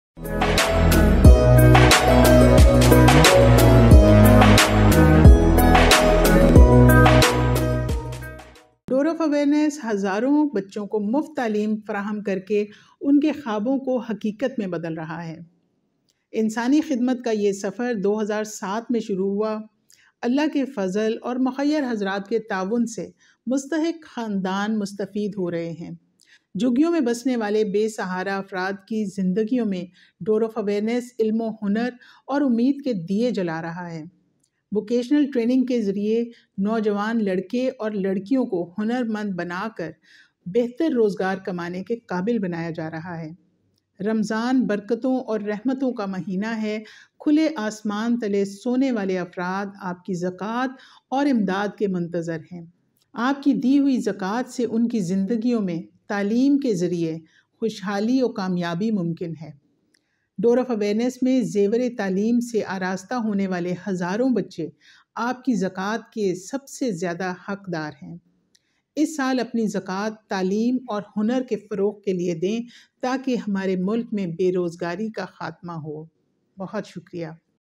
Mrs Ruba Humayun appealed to all Muslims for support DOA education system .
Mrs Ruba Humayun Chairperson Door of awareness appealed to all Muslims for support DOA education system which are changing life of underprivileged children and families. Give your Zakat to Door of awareness for betterment of Society.

For Online Transaction:
Account Title: Door of Awareness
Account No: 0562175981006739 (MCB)